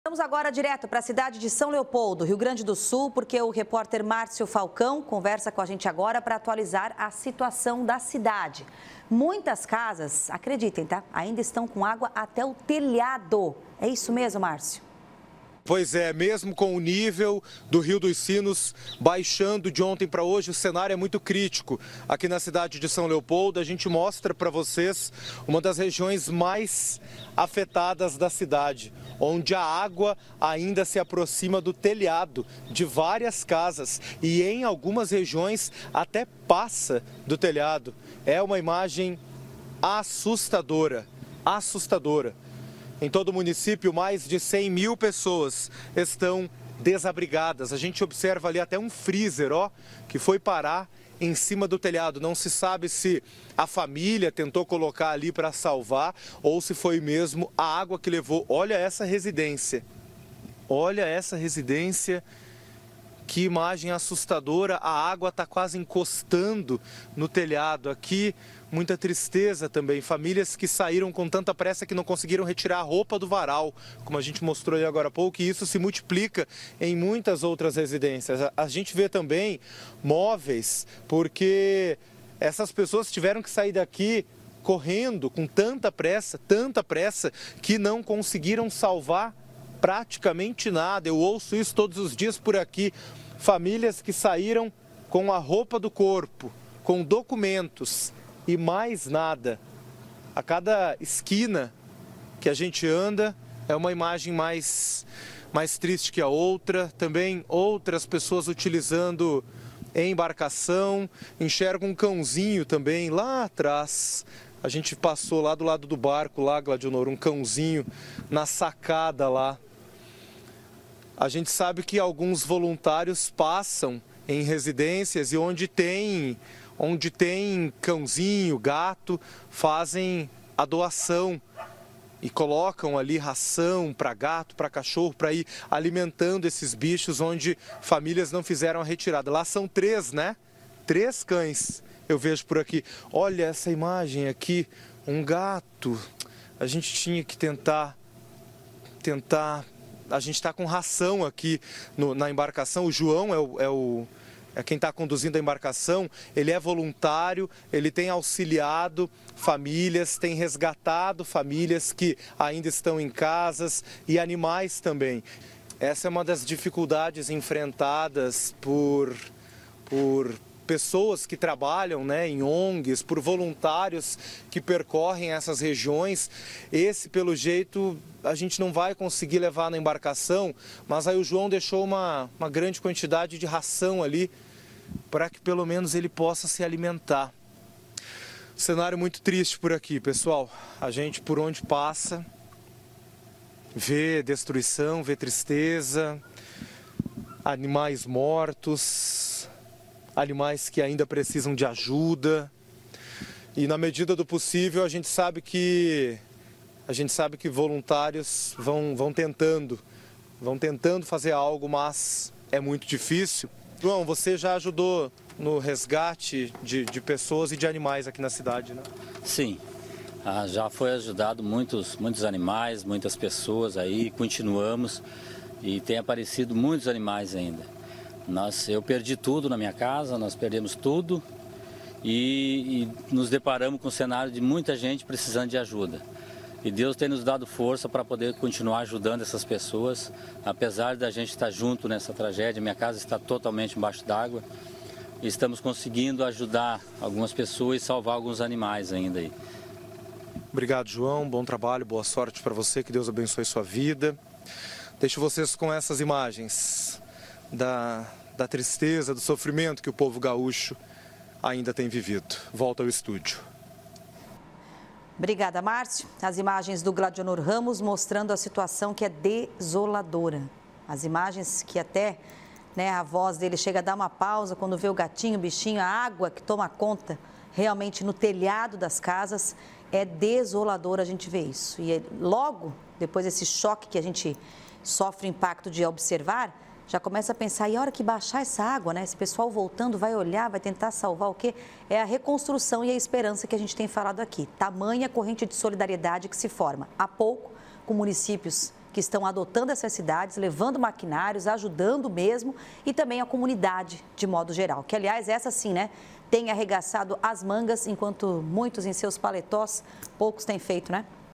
Nível do Rio dos Sinos começa a baixar, mas água ainda chega até o telhado de muitas casas
"Depois de dias de intensa preocupação, o nível do Rio dos Sinos finalmente começa a baixar, trazendo um alívio parcial para os moradores da região. No entanto, a situação ainda é crítica. Em muitas áreas, a água permanece alta, chegando até o telhado das casas, deixando várias famílias desabrigadas e enfrentando perdas significativas. Equipes de resgate e voluntários continuam trabalhando incansavelmente para prestar socorro e fornecer assistência às vítimas. A prioridade agora é garantir a segurança dos moradores e começar os esforços de limpeza e recuperação das áreas afetadas. A comunidade local, assim como diversas entidades de apoio, estão mobilizadas para enfrentar os desafios que ainda persistem."